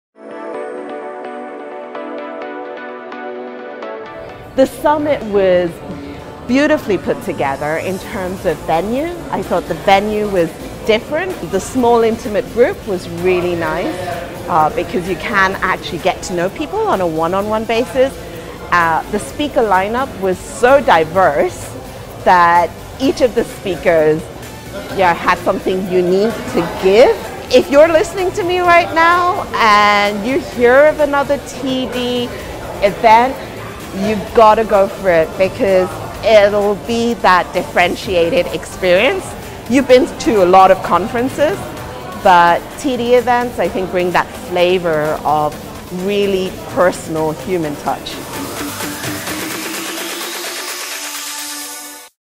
Sushma Panikker Ceaser - HR Executive Summit
"You've been to a lot of conferences, but TDevents bring that flavour of really personal human touch" — Sushma Panikker Ceasar, former leadership and talent development director of Emaar Hospitality Group, tells of her experience at the TDHRsummit!